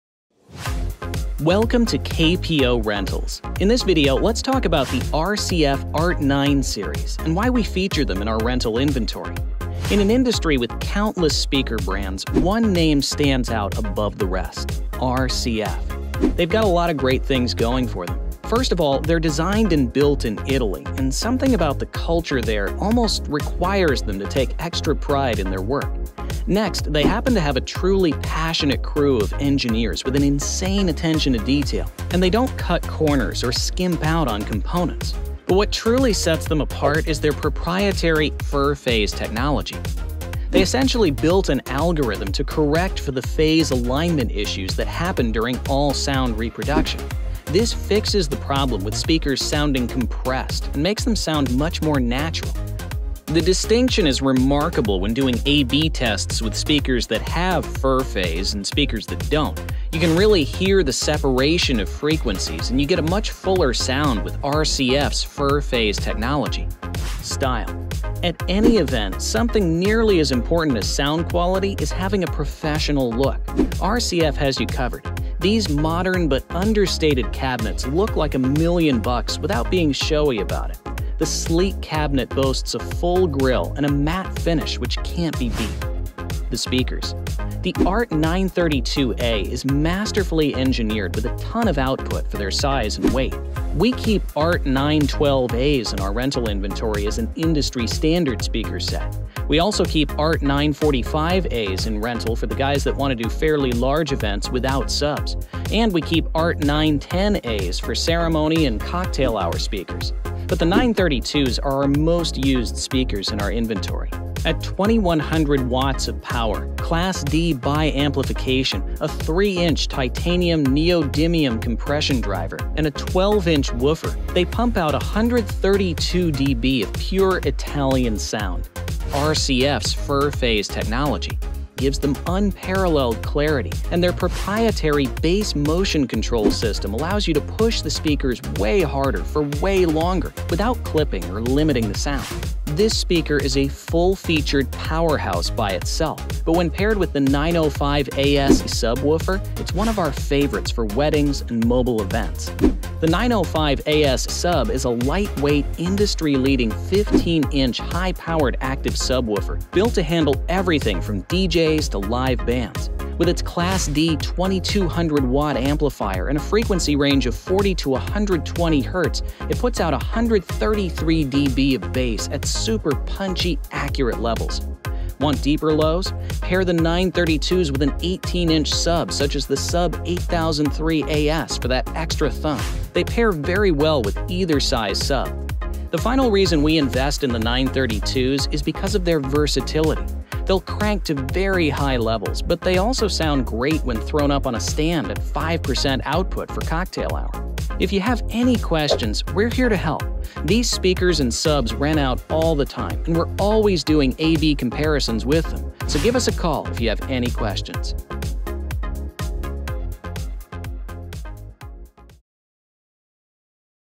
Our #1 Audio Rentals: RCF ART 932A & Sub 905A MK3
RCF ART 932A

The RCF ART series is our both our top selling and top renting speaker line. Excellent quality from a brand you can trust!

Save ~ 22% on Audio & Lighting Gear!

"Better Deals without the Hassle" is our slogan.